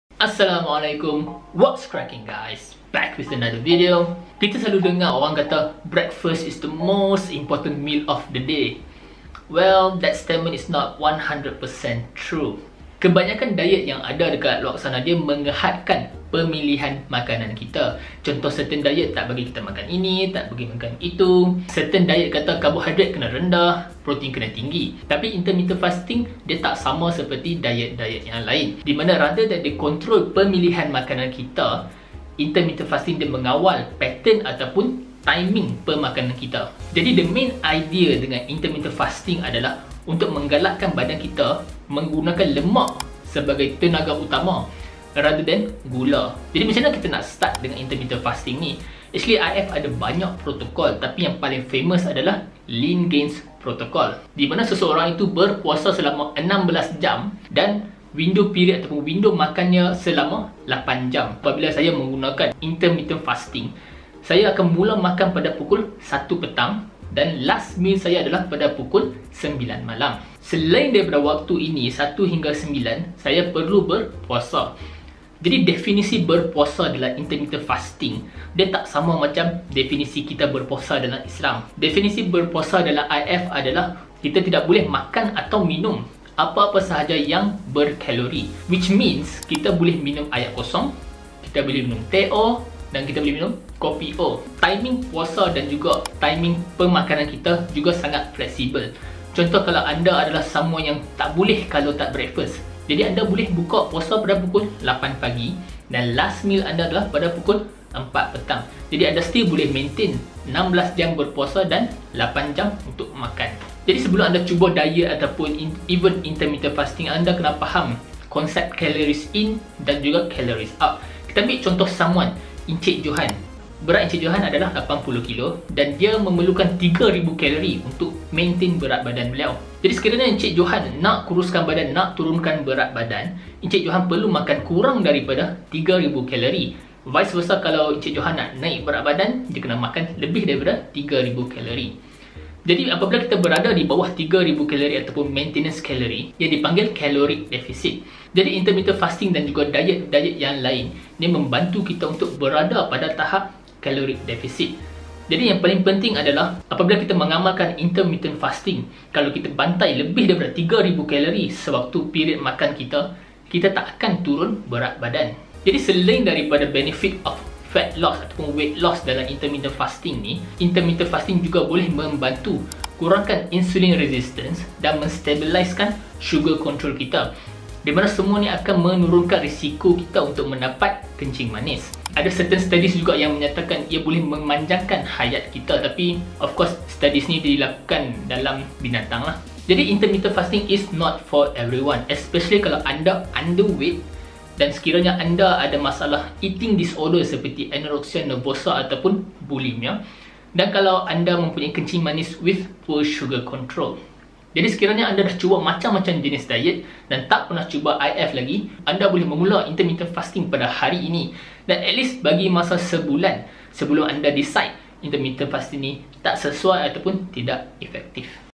Apa Itu Intermittent Fasting?
Intermittent Fasting adalah satu lagi tool atau diet yang boleh sering digunakan oleh ramai ahli bina badan dan juga pelakon kerana hasil yang mampu di capai menggukana intermittent fasting dengan betul.